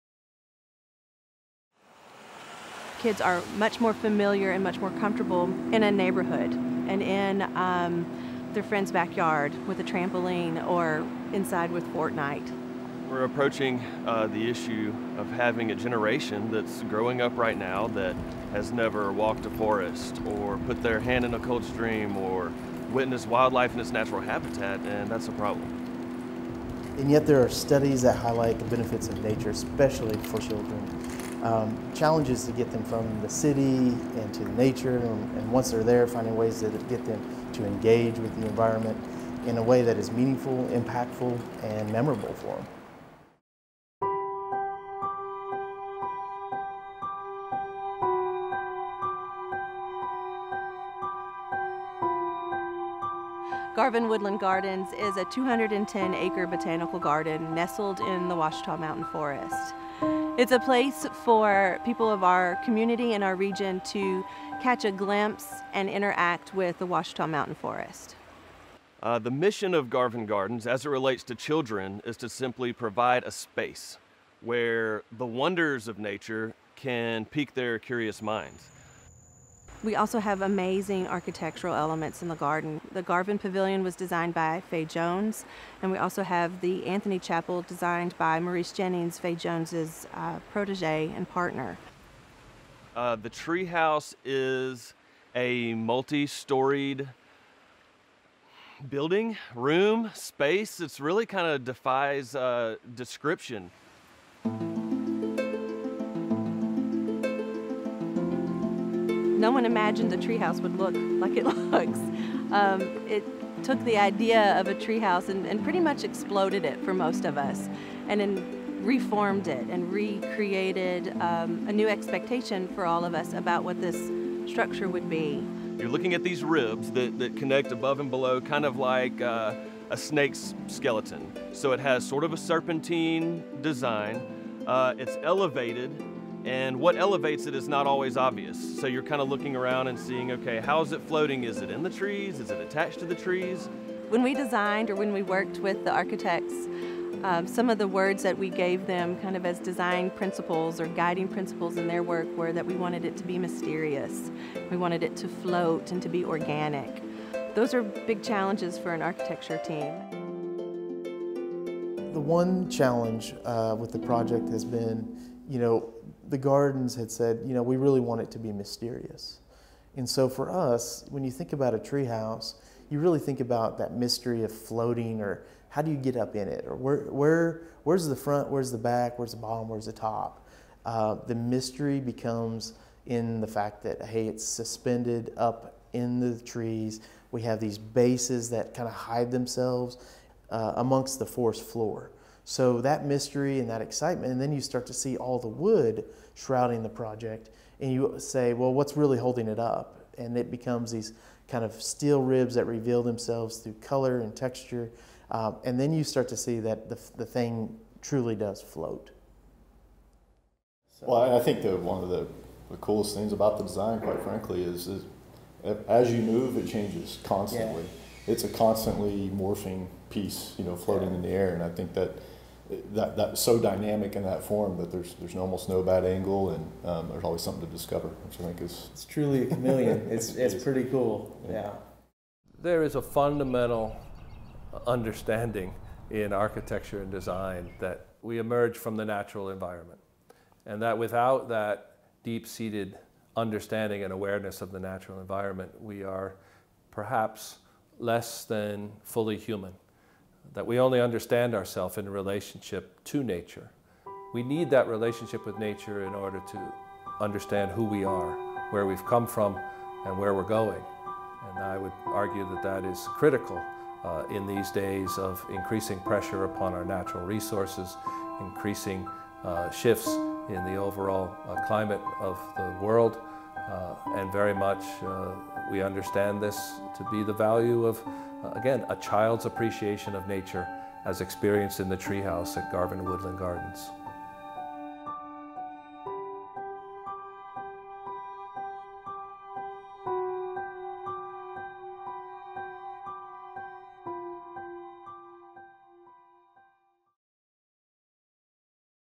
The Tree House: Reconnecting Kids With Nature
With the migration from rural to urban environments, it's becoming more difficult to engage kids with their natural environment. Kids are growing up alienated from nature and without access to it. "The Tree House" is about the efforts of Garvan Woodland Gardens in Hot Springs, Arkansas to create something that will spark the imagination of the children who visit the park and help them explore nature through innovative architecture and design.

Producer/Editor:
Mark Wilcken

Videography:
Gerard Matthews, Hot Springs
Cannon McNair, Fayetteville

Additional Video:
Garvan Woodland Gardens
61 Celsius
Modus Studio
Kyle Schraeder

Special Thanks:
Sherre Freeman
Lane Schmidt
Michelle Parks
Michael Adam Martz
Chris Lankford
The University of Arkansas &
The Fay Jones School of Architecture and Design

Architectural Firm:
Modus Studio